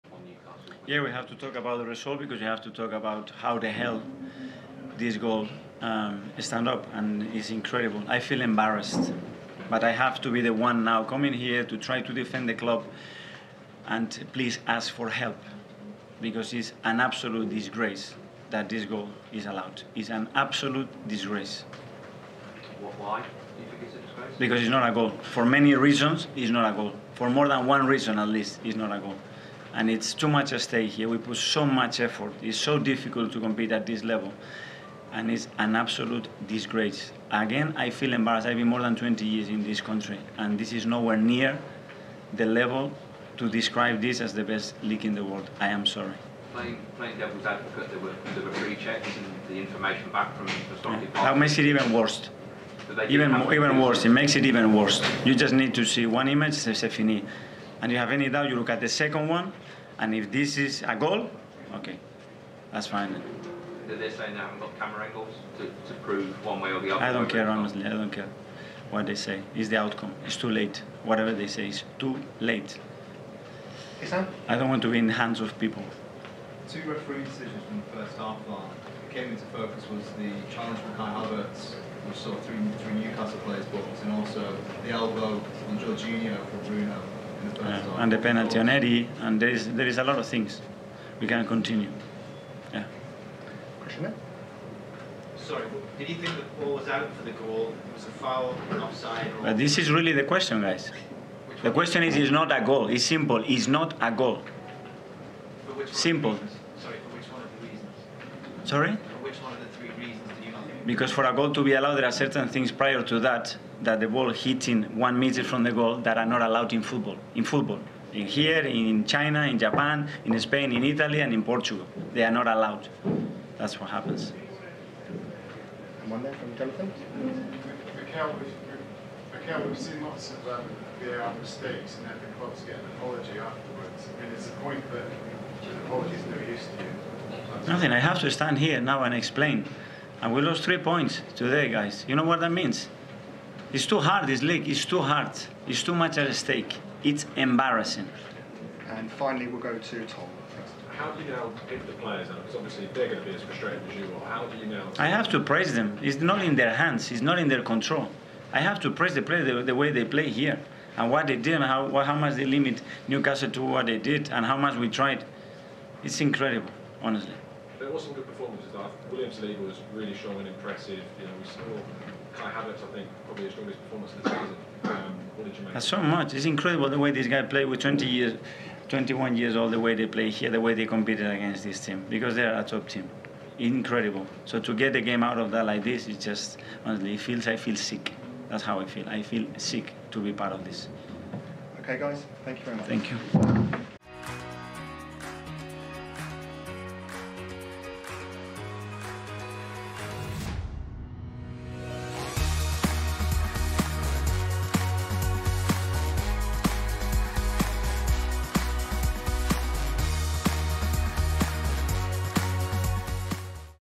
'I feel sick.' 🤢 | Arteta SLAMS VAR decision in Arsenal defeat to Newcastle
Arsenal manager Mikel Arteta slams the decision to allow Anthony Gordon's goal to stand in Newcastle's 1-0 win over the Gunners in the Premier League.